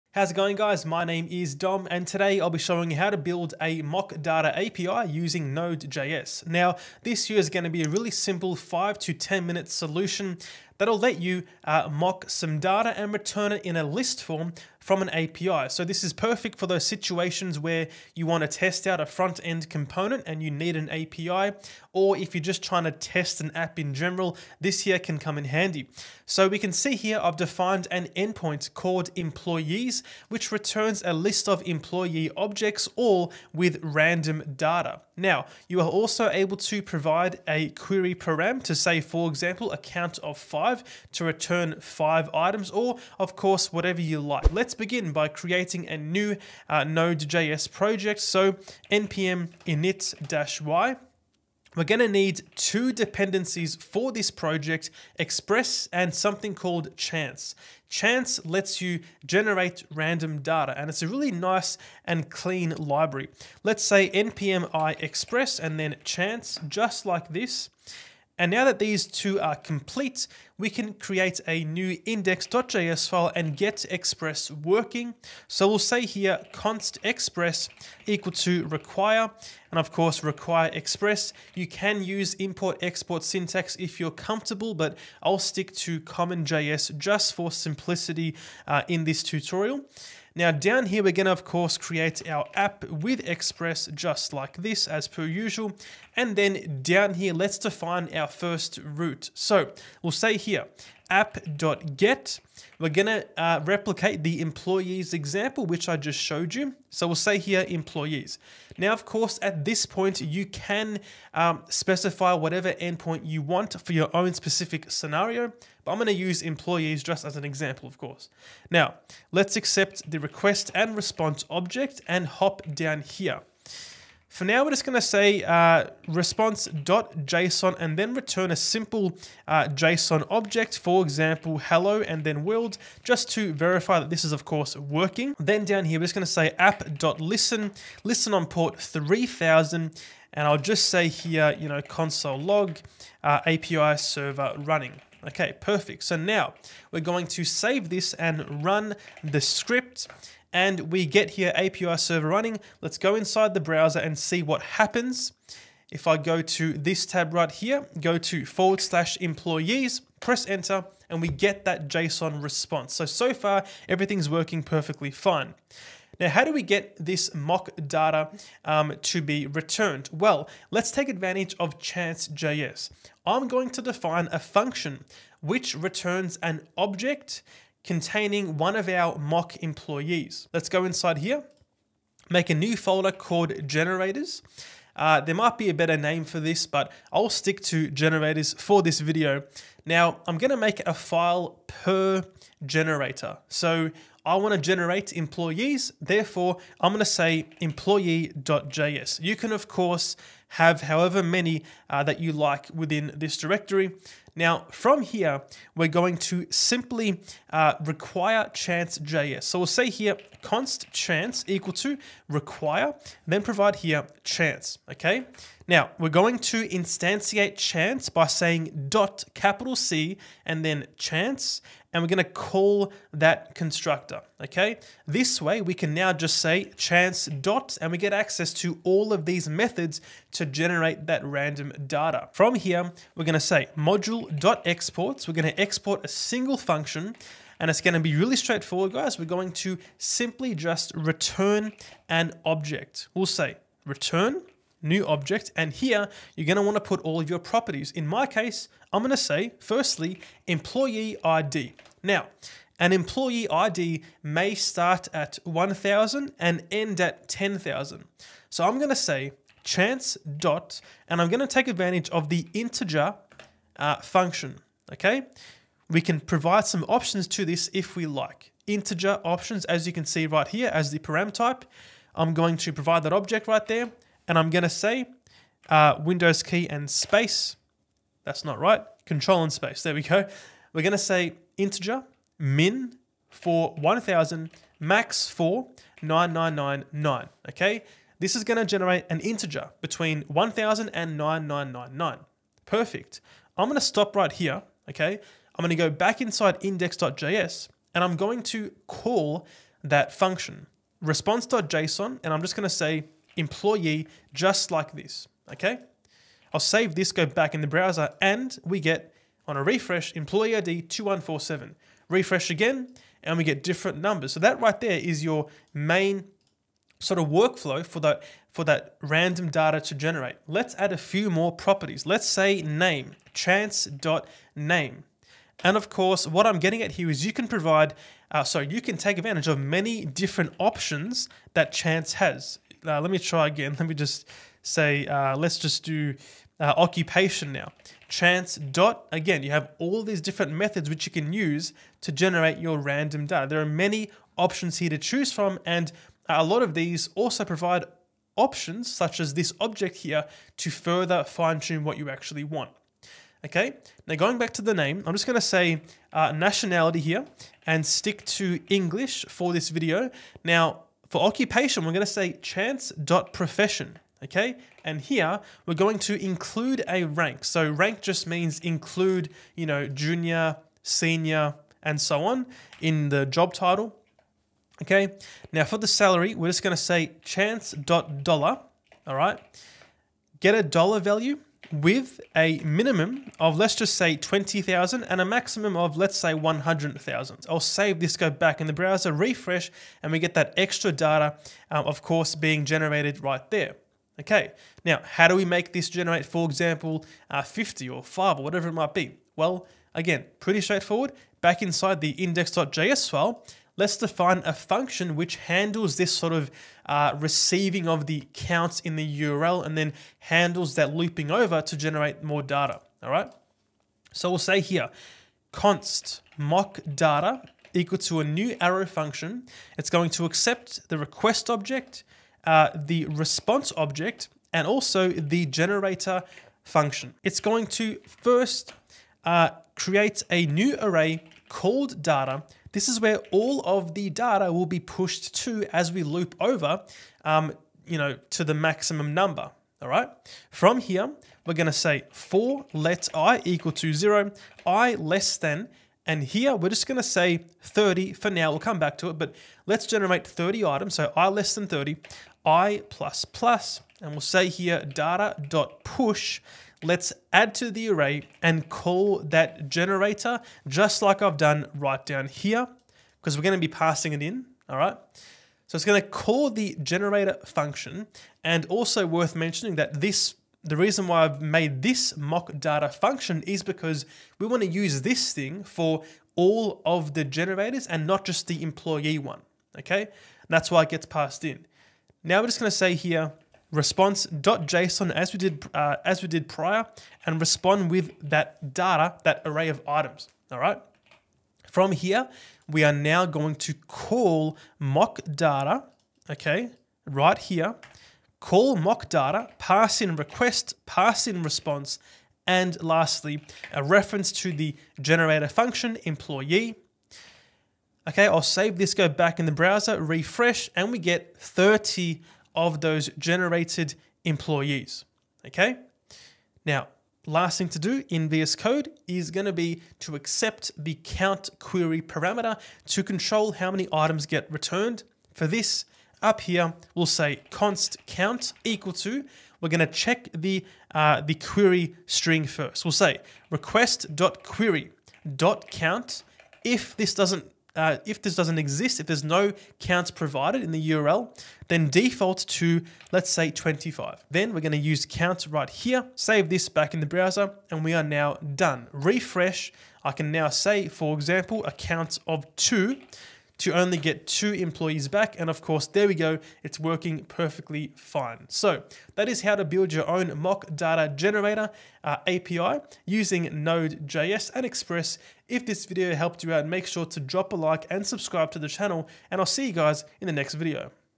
Tired Of Hard Coding Data? Build a Mock Data API with Node.js
Let's have a look at how easy it is to build an API which returns lists of mock data - perfect for testing apps or UI components 😎